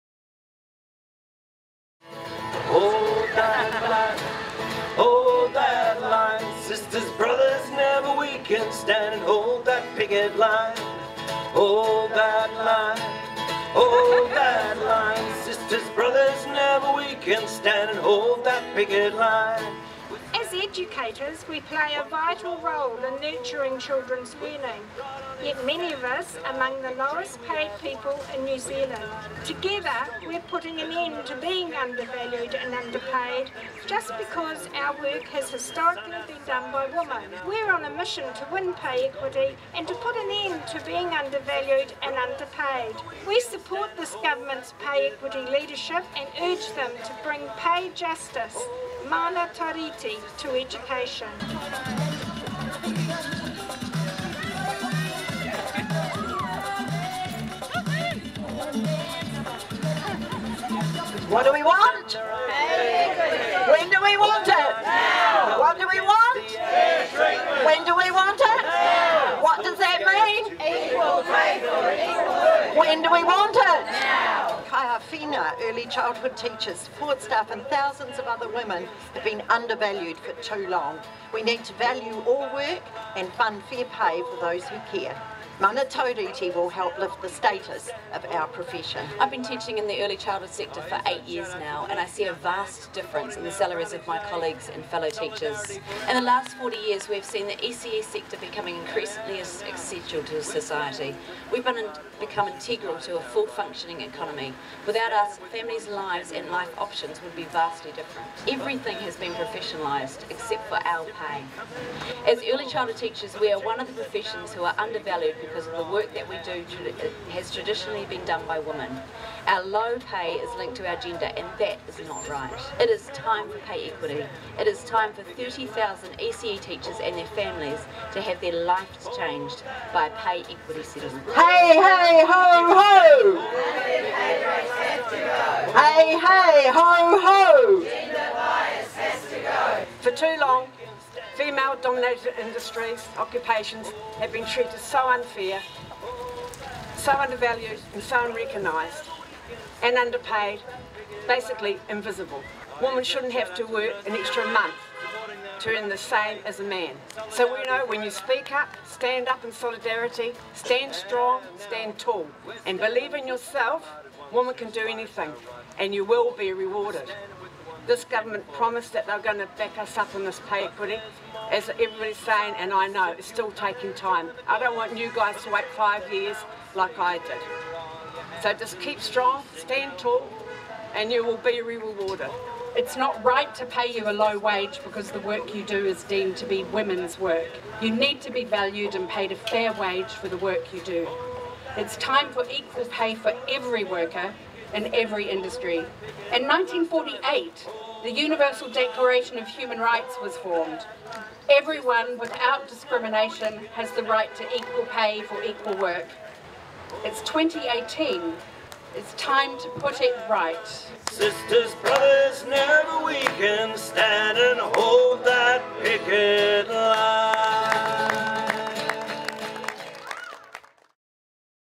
NZEI Fair's Fair event - Palmerston North, NZ
NZEI's Fair's Fair event promoting equal pay for equal work.

This event was held in The Square, Palmerston North in 2018.